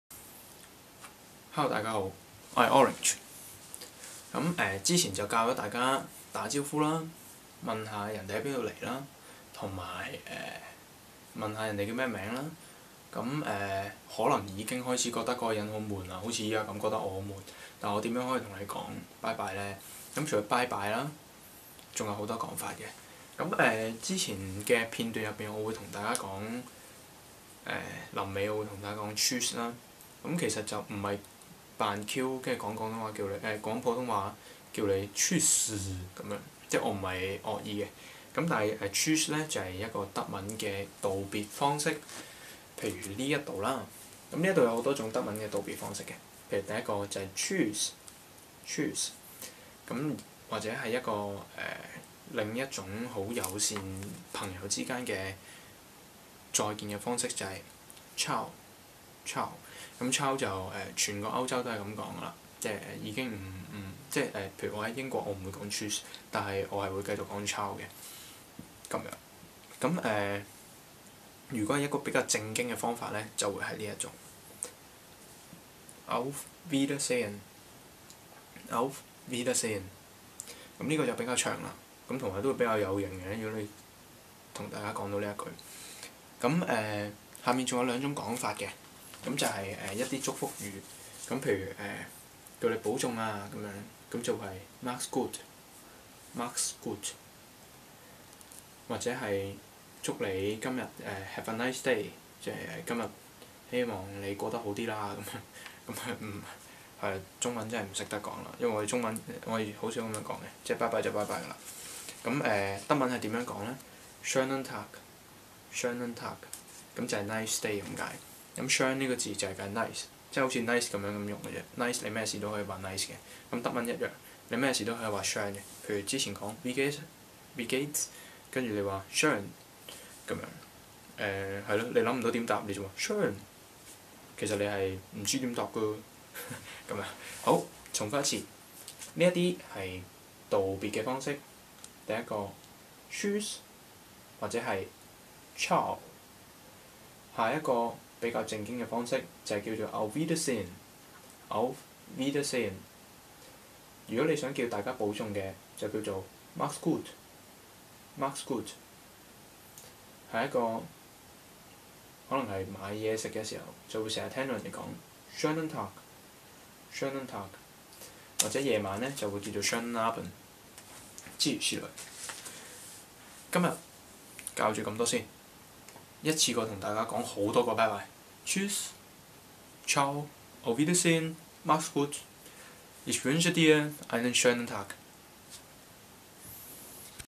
第4課 道別
是日重點: 道別

再說再見，再講拜拜?
1. Ciao
2. tschüß
3. Auf Wiedersehen

保重!
-- Macht's gut!

Have a nice day! // Have a nice evening
-- Schönen Tag // Schönen Abend

名堂: Oranch // 香港浸會大學 // 歐洲研究生 (德文系) // 三年級 [ 從事如何有效地發揚「自己」的效究工作 ]

技能: 學德文2年 // 戰鬥力: C1

任務: 用德文教廣東話 & 用廣東話教德文

好好享受!! 你的耐性，是最好的回報!!

事後研究:

1) Facebook : Oranch Naranja Cheung //